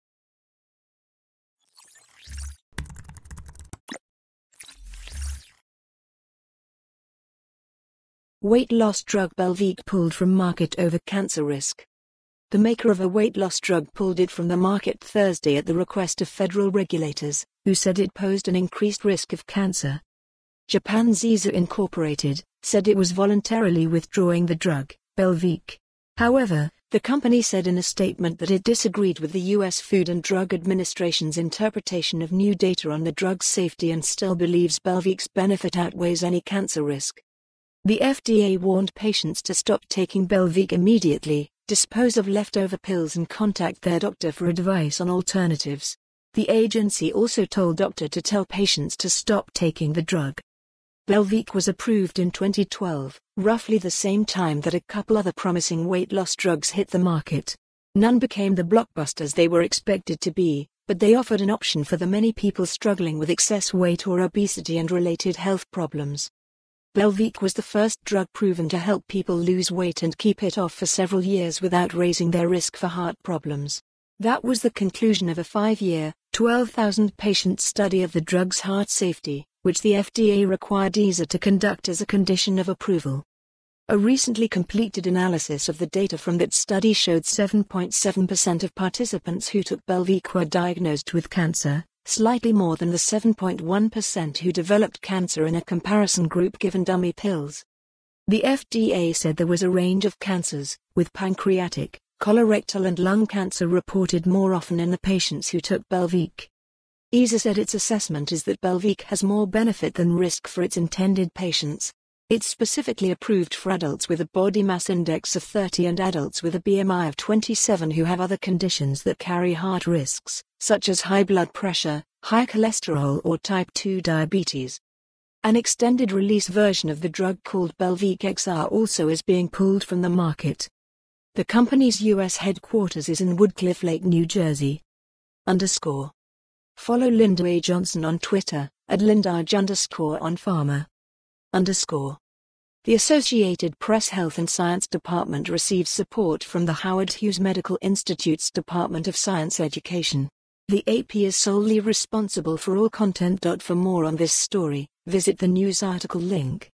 Weight loss drug Belviq pulled from market over cancer risk
The maker of a weight loss drug pulled it from the market Thursday at the request of federal regulators, who said it posed an increased risk of cancer.

Japan's Eisai Inc., said it was voluntarily withdrawing the drug, Belviq. However, the company said in a statement that it disagreed with the U.S. Food and Drug Administration's interpretation of new data on the drug's safety and still believes Belviq's benefit outweighs any cancer risk.

The FDA warned patients to stop taking Belviq immediately, dispose of leftover pills and contact their doctor for advice on alternatives. The agency also told doctor to tell patients to stop taking the drug.

Belviq was approved in 2012, roughly the same time that a couple other promising weight loss drugs hit the market. None became the blockbusters they were expected to be, but they offered an option for the many people struggling with excess weight or obesity and related health problems.

Belviq was the first drug proven to help people lose weight and keep it off for several years without raising their risk for heart problems. That was the conclusion of a five-year, 12,000-patient study of the drug's heart safety, which the FDA required Eisai to conduct as a condition of approval.

A recently completed analysis of the data from that study showed 7.7% of participants who took Belviq were diagnosed with cancer, slightly more than the 7.1% who developed cancer in a comparison group given dummy pills.

The FDA said there was a range of cancers, with pancreatic, colorectal and lung cancer reported more often in the patients who took Belviq.

Eisai said its assessment is that Belviq has more benefit than risk for its intended patients. It's specifically approved for adu